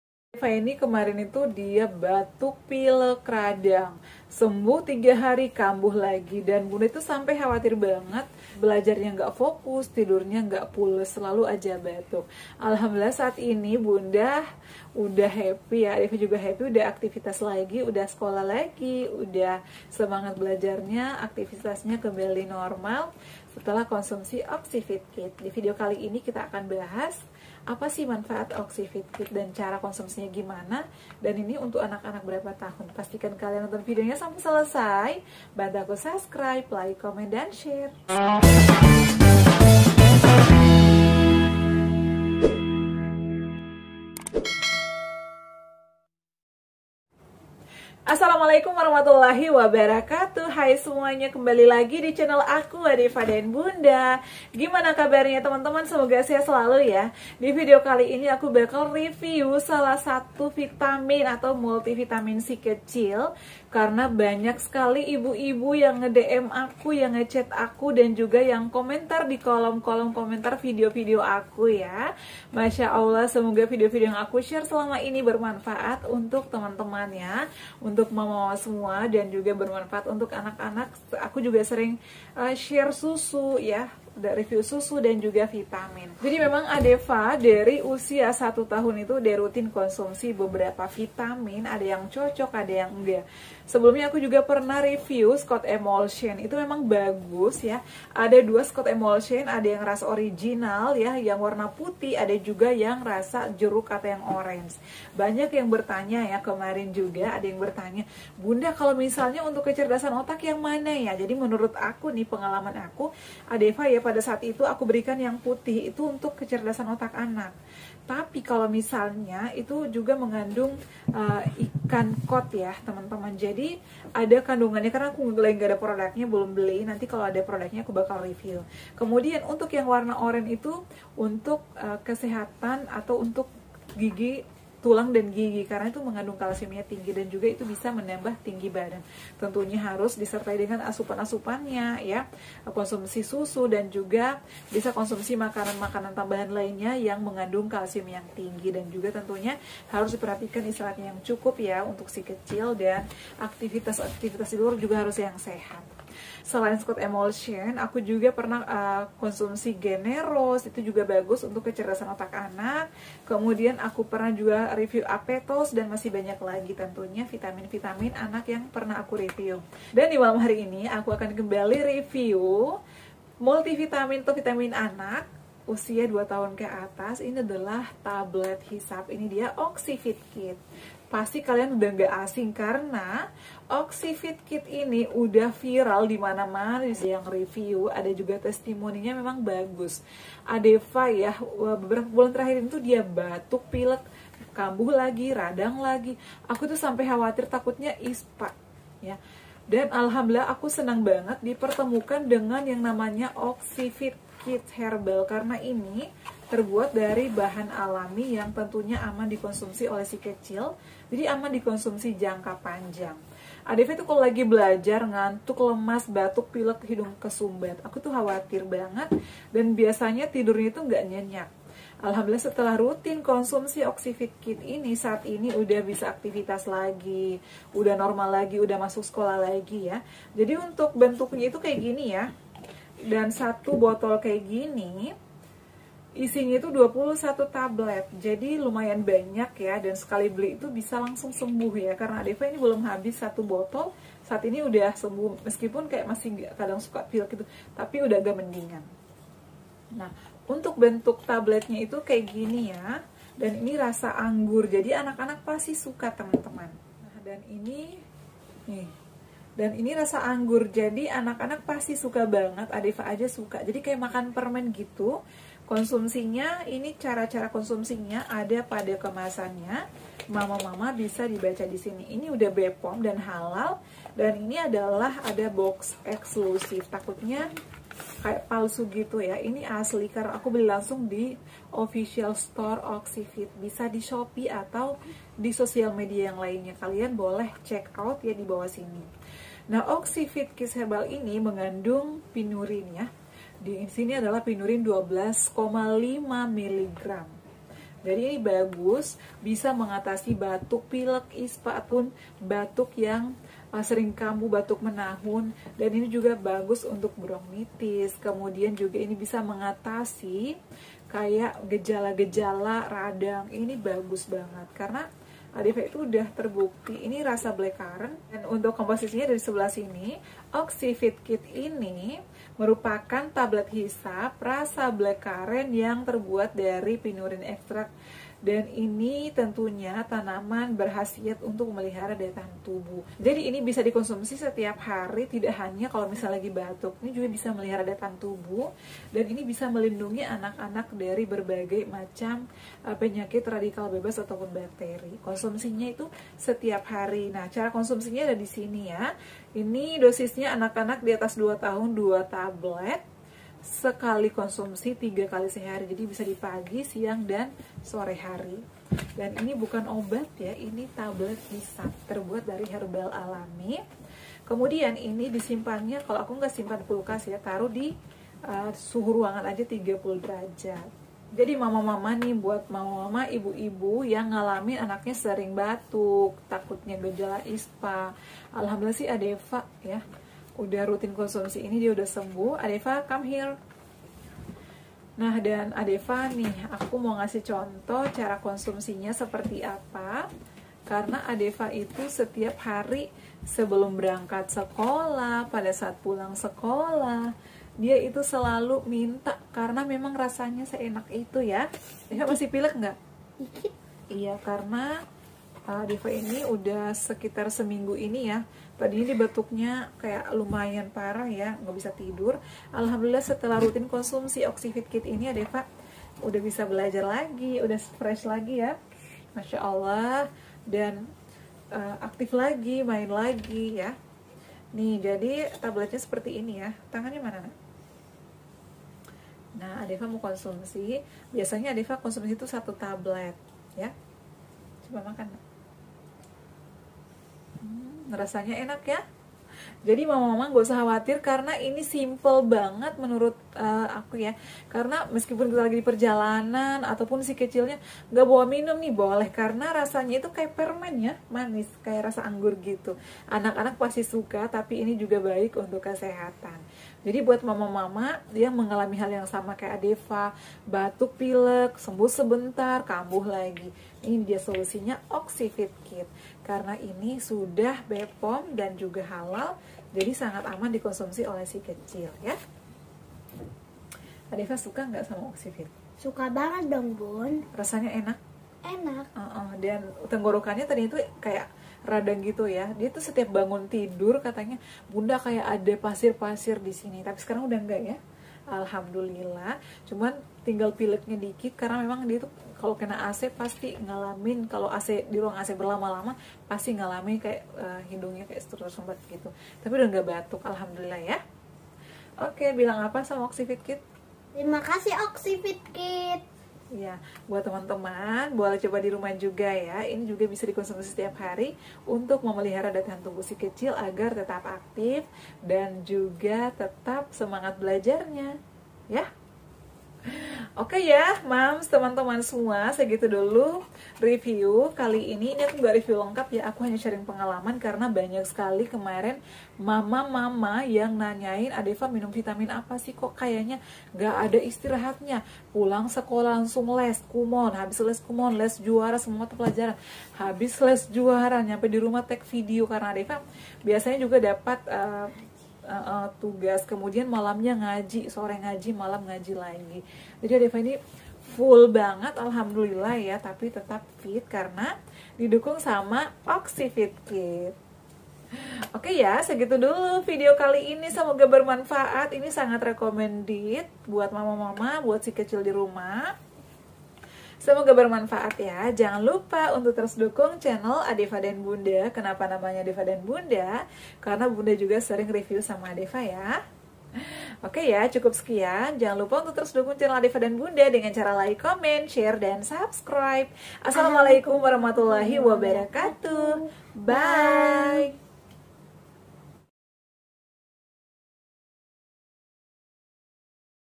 REKOMENDASI VITAMIN ANAK UNTUK MEMELIHARA DAYA TAHAN TUBUH ~ REVIEW OXYVIT KIDS
Assalamualaikum, hallo semuanya kali ini aku review dan share pengalaman setelah Adeeva konsumsi vitamin herbal Oxyvitkids Alhamdulillah cocok, saat ini Adeeva sudah bisa sekolah dan melakukan aktivitas yang lainnya. Buat mam yang anaknya mengalami hal yang sama seperti Adeeva Yukk cobain oxyvitkids sekarang juga.